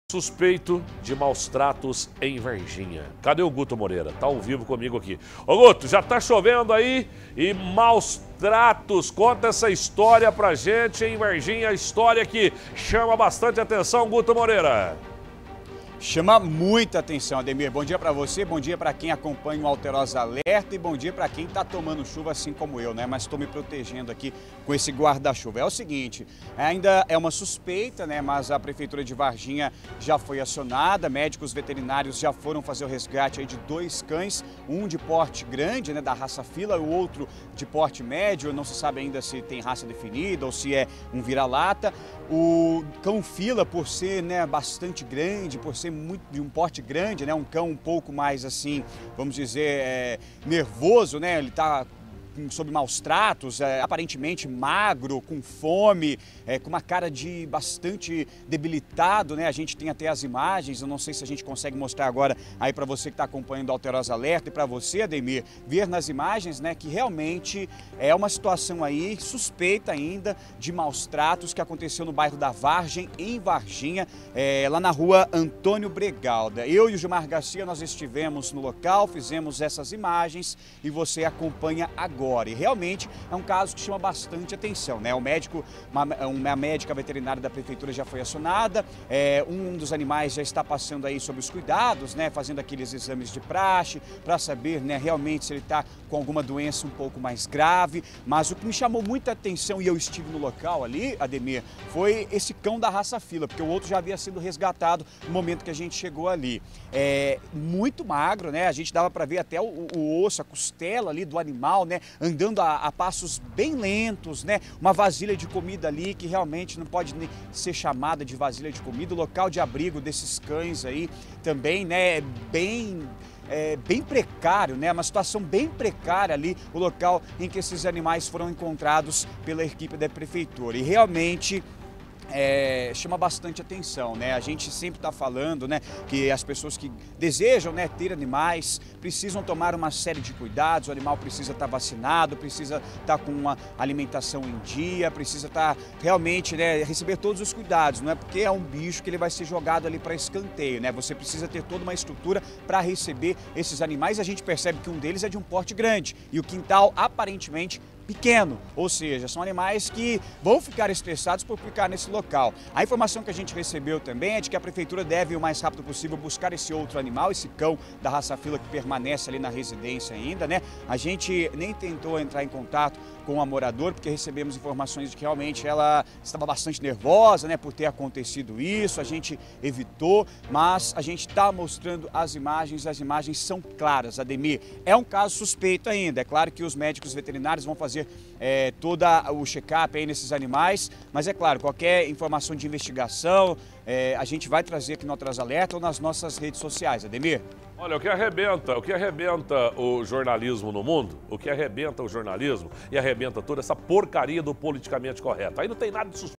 Cachorros resgatados após maus-tratos em Varginha
CACHORROS RESGATADOS APÓS MAUS-TRATOS | Cães em situação de maus-tratos foram resgatados em Varginha.